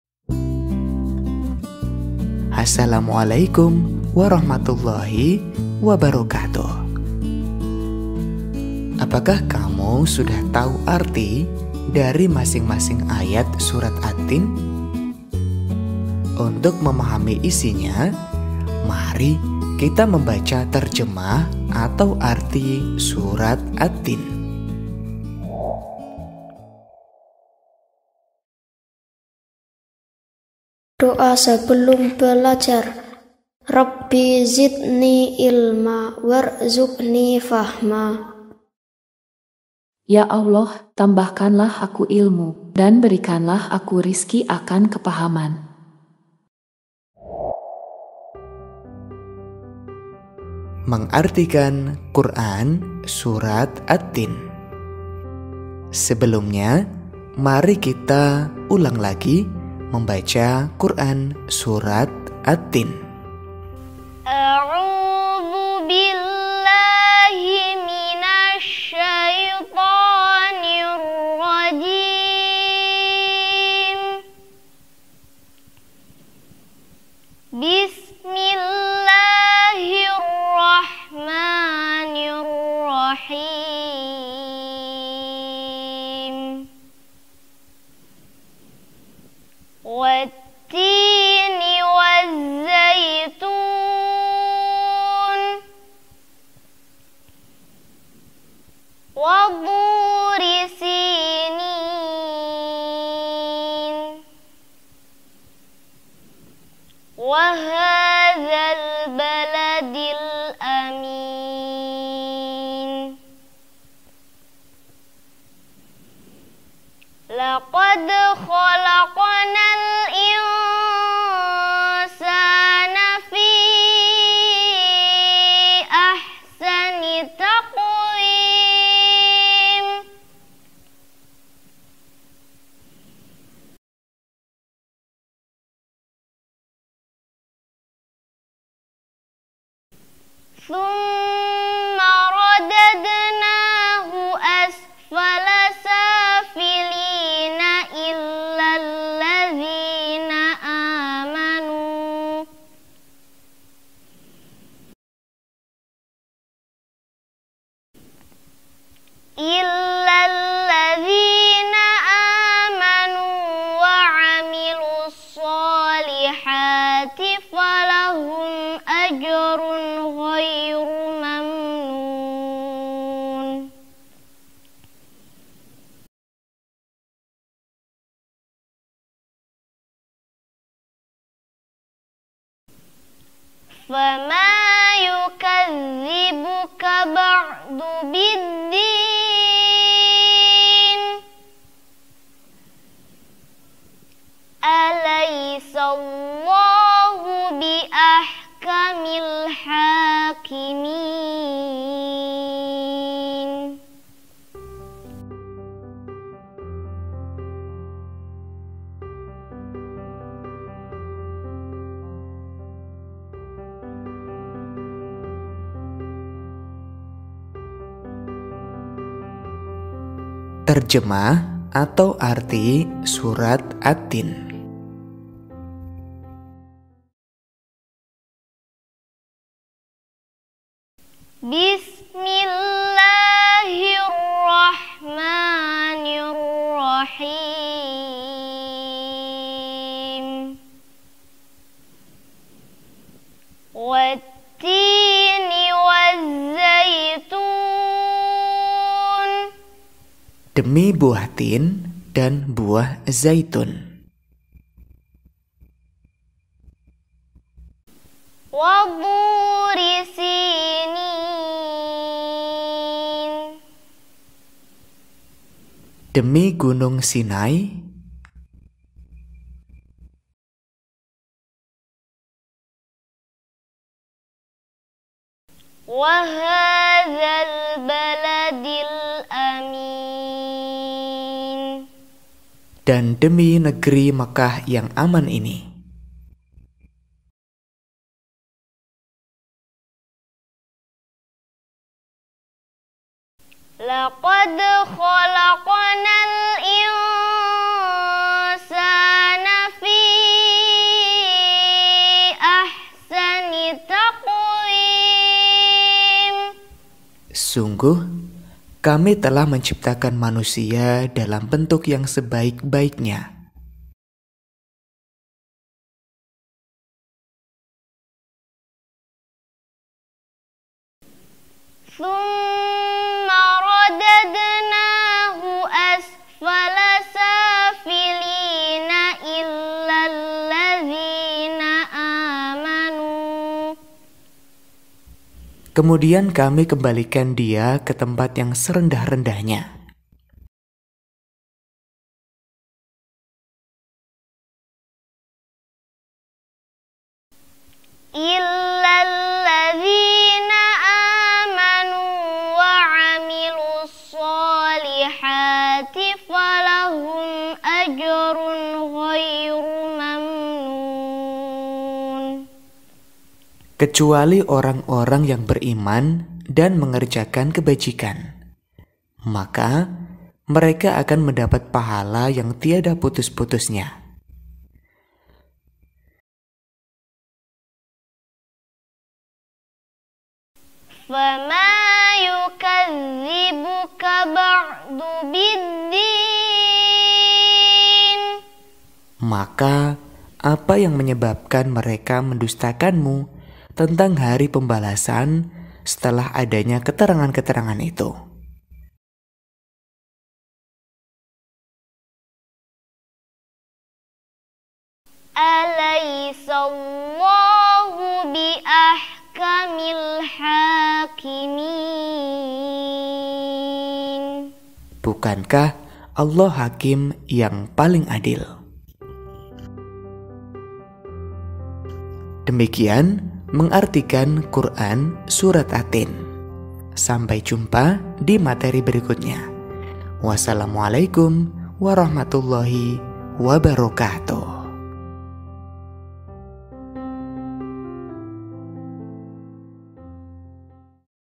Materi PAI Kelas 4 Kurikulum Merdeka Bab 6 | Mengartikan Surat At-Tiin
Mari kita belajar seputar Pengetahuan islam dan Materi Pelajaran PAI dan Budi Pekerti dengan menyenangkan melalui video berbasis animasi whiteboard (White Board Animation) Materi PAI Kelas 4 Kurikulum Merdeka Bab 6 | Mengartikan Surat At-Tiin

Editor : Lisanto, S.Pd.I
Materi : Vera Andriana, S.Pd.